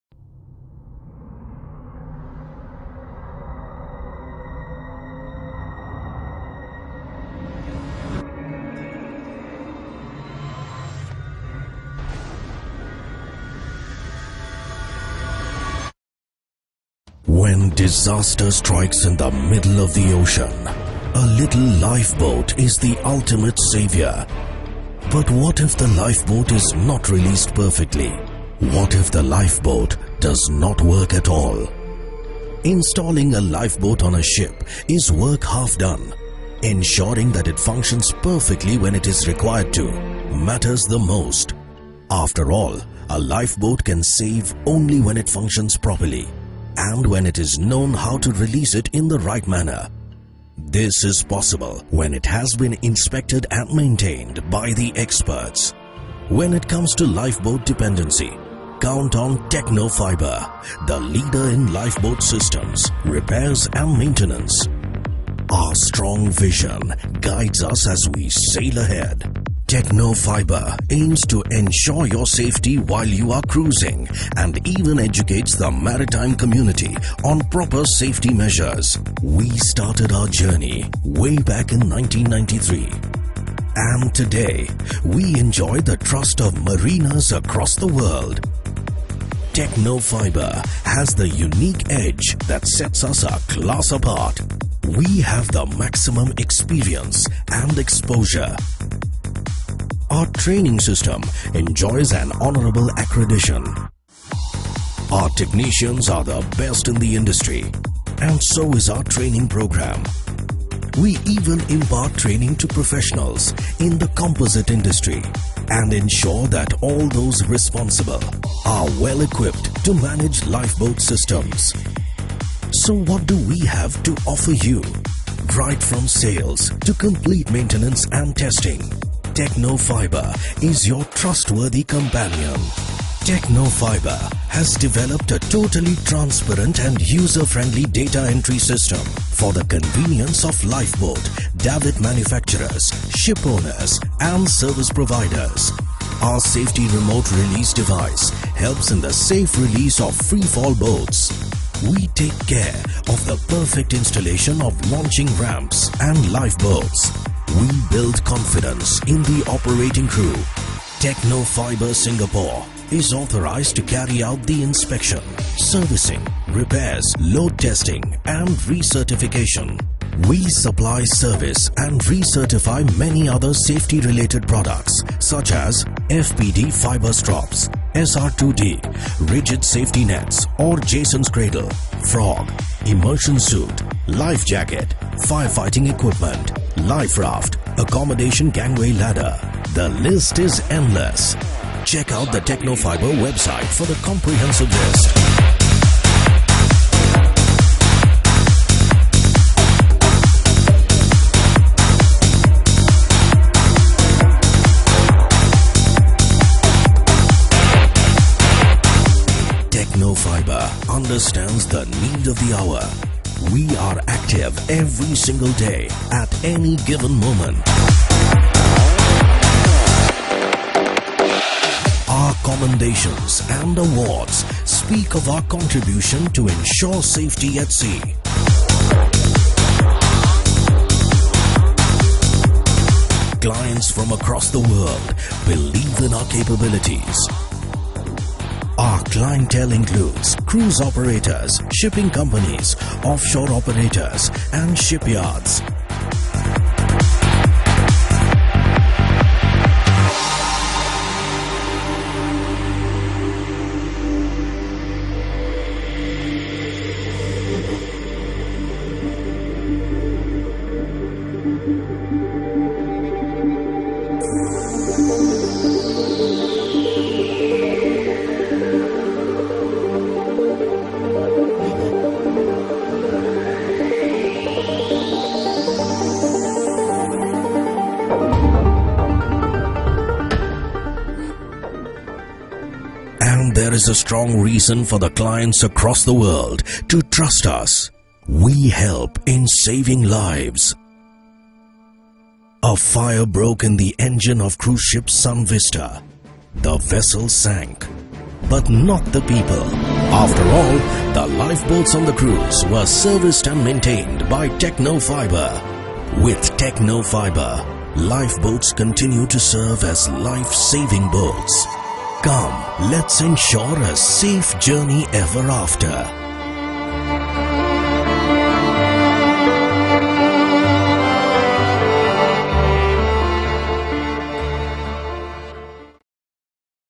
TechnoFIBRE Corporate Video
TechnoFIBRE- serving MarineSafety Internationally
Maintenance, Repair, Overhaul, Testing and Training of Lifeboats, Davits, Winches and other marine related products and services.